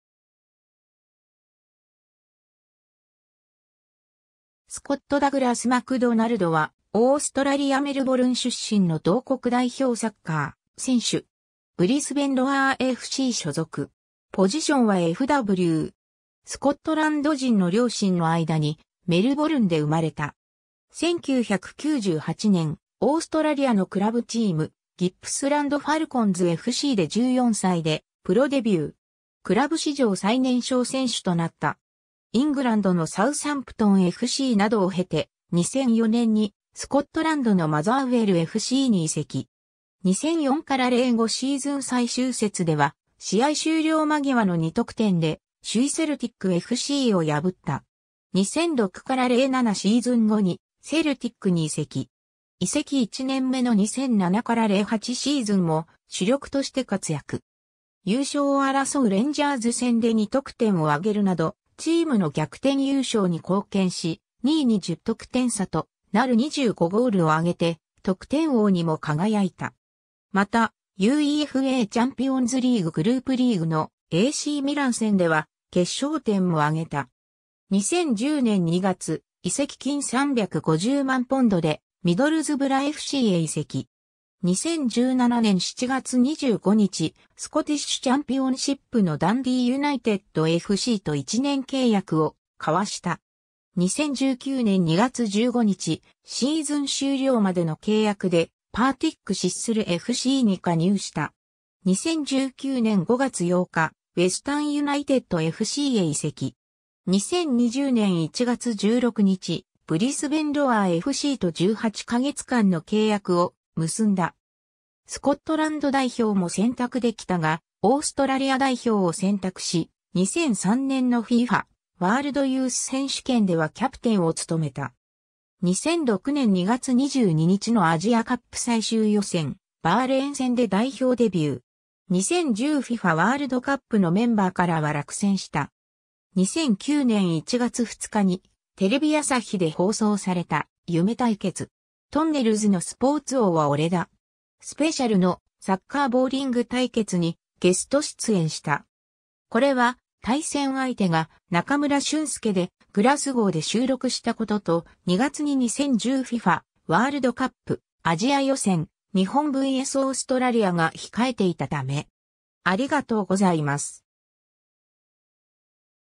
スコット・マクドナルド
ハダースフィールド・タウンFCの選手
スコット・ダグラス・マクドナルド（Scott Douglas McDonald, 1983年8月21日 - ）は、オーストラリア・メルボルン出身の同国代表サッカー選手。
ブリスベン・ロアーFC所属。
 スコットランド人の両親の間にメルボルンで生まれた。
1998年、オーストラリアのクラブチーム、ギップスランド・ファルコンズFCで14歳でプロデビュー。
クラブ史上最年少選手となった。
 イングランドのサウサンプトンFCなどを経て、2004年にスコットランドのマザーウェルFCに移籍。
2004-05シーズン最終節では、試合終了間際の2得点で首位セルティックFCを破った（その結果、最終節で勝利したレンジャーズFCが勝ち点1差で逆転優勝を決めた）。
 2006-07シーズン後にセルティックに移籍。
移籍1年目の2007-08シーズンも主力として活躍。
優勝を争うレンジャーズ戦で2得点を挙げるなどチームの逆転優勝に貢献し、2位に10得点差となる25ゴールを挙げて得点王にも輝いた。
また、UEFAチャンピオンズリーグ・グループリーグのACミラン戦では決勝点もあげた。
2010年2月、移籍金350万ポンドでミドルズブラFCへ移籍。
 2017年7月25日、スコティッシュ・チャンピオンシップ（2部相当）のダンディー・ユナイテッドFCと1年契約を交わした。
 2019年2月15日、シーズン終了までの契約でパーティック・シッスルFCに加入した。
 2019年5月8日、ウェスタン・ユナイテッドFCへ移籍。
 2020年1月16日、ブリスベン・ロアーFCと18か月間の契約を結んだ。
 スコットランド代表も選択できたがオーストラリア代表を選択し、2003年のFIFAワールドユース選手権ではキャプテンを務めた。
2006年2月22日のアジアカップ最終予選・バーレーン戦で代表デビュー。
2010 FIFAワールドカップのメンバーからは落選した。
 2009年1月2日にテレビ朝日で放送された「夢対決!とんねるずのスポーツ王は俺だ!スペシャル」のサッカーボウリング対決にゲスト出演した。
これは対戦相手が中村俊輔でグラスゴーで収録したことと、2月に2010 FIFAワールドカップ・アジア予選・日本vsオーストラリアが控えていたため。